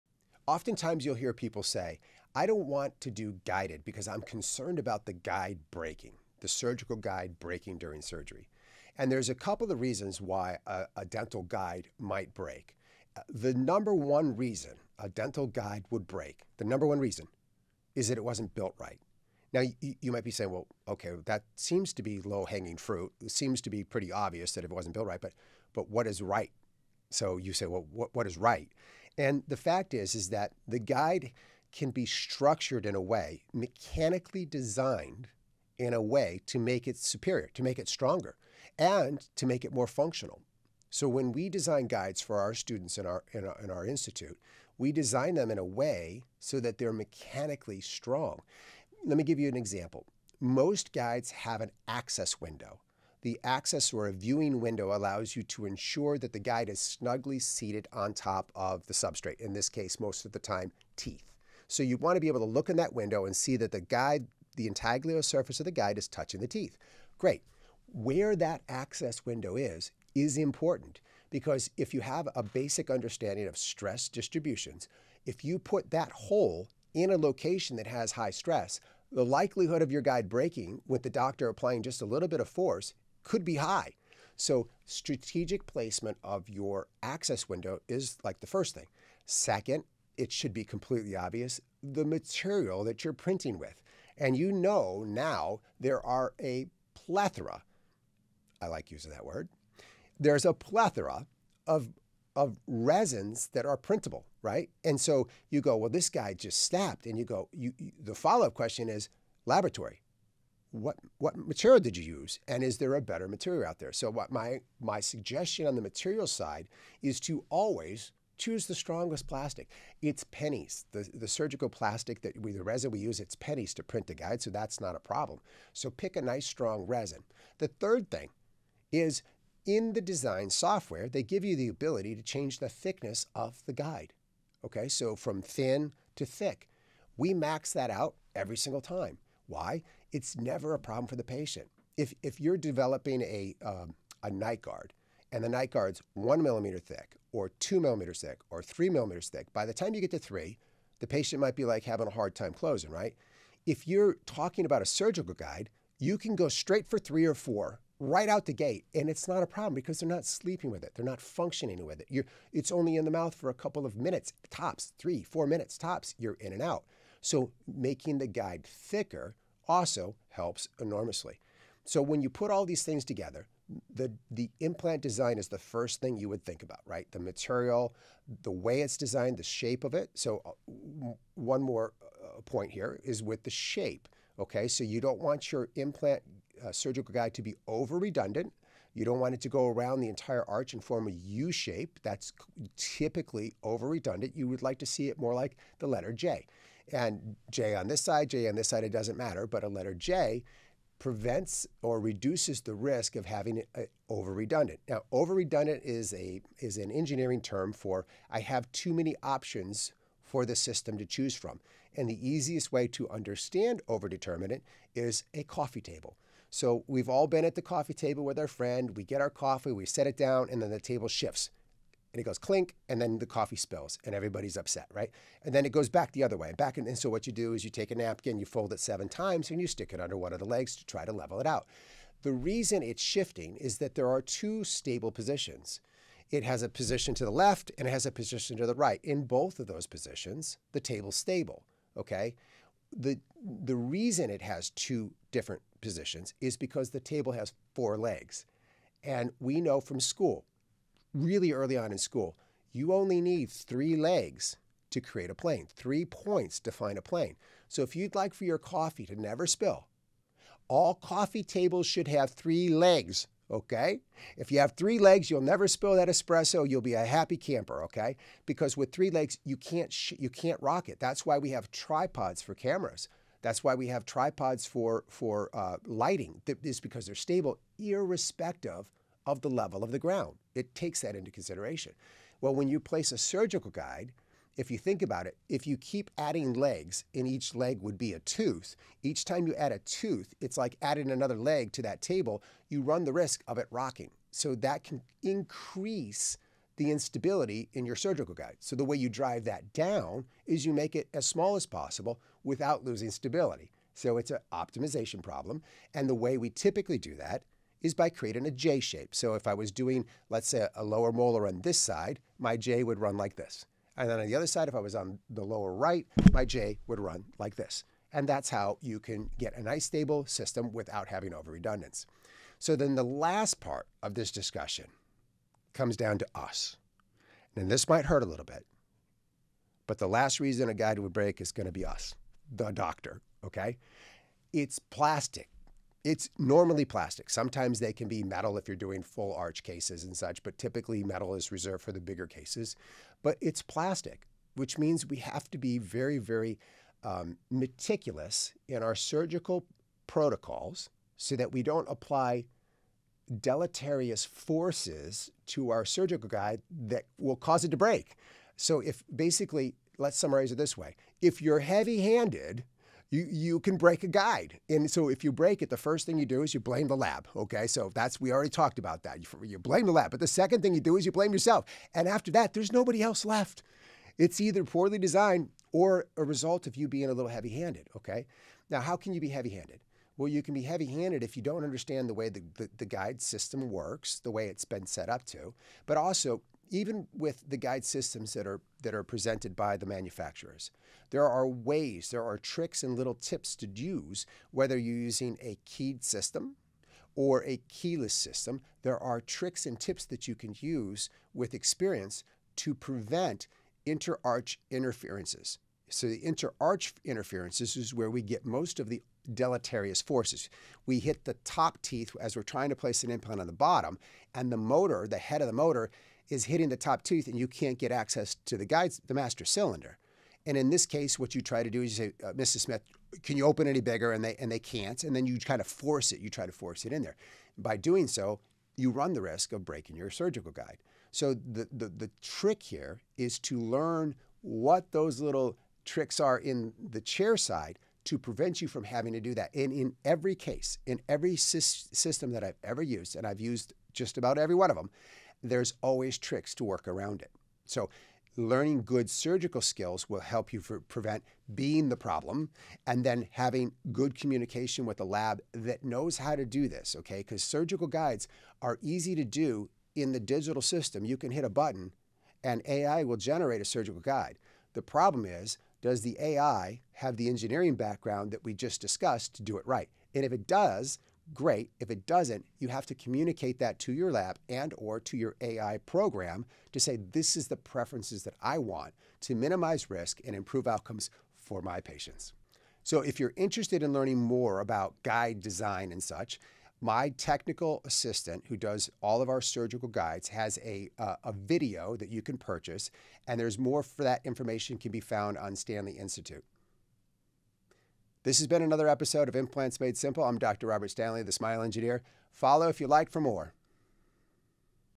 If Your Surgical Guides Fail, THIS Is Why
In this week’s video, Dr. Robert Stanley dives deep into the mechanics and materials behind surgical guide design so you can finally understand why guides fail and how to make them work reliably every time. Whether you’re a clinician or a lab partner, this breakdown from a lab perspective will change how you plan, design, and troubleshoot your guided cases. If you’ve ever struggled with inconsistent guide performance, this is the video for you. Learn how better design, smarter material choices, and thoughtful engineering can elevate your guided surgery outcomes.

Are you a patient looking for our dental practice?